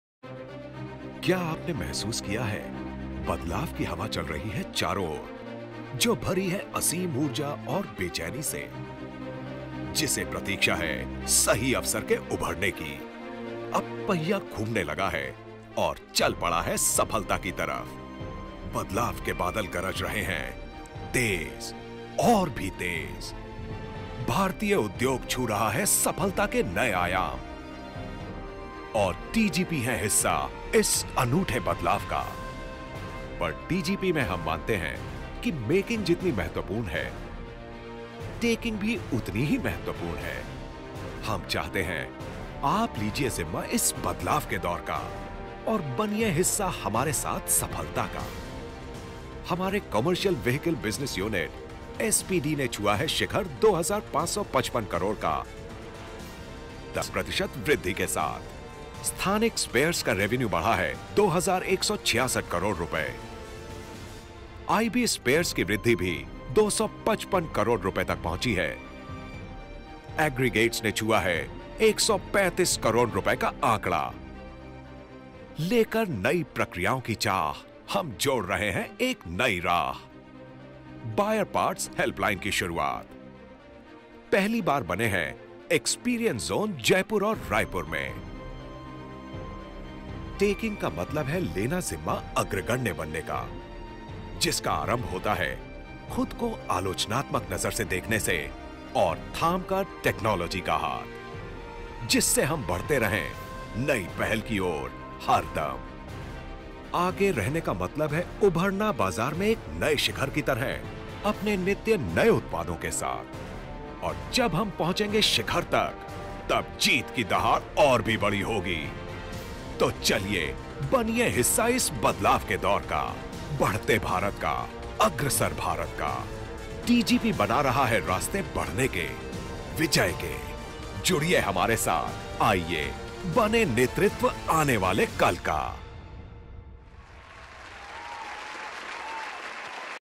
Tata Genuine Parts Annual Distributors Conference Curtain Raiser - Beijing
Tata Genuine Parts Annual Distributors Conference Curtain Raiser - Held at Beijing in the year 2016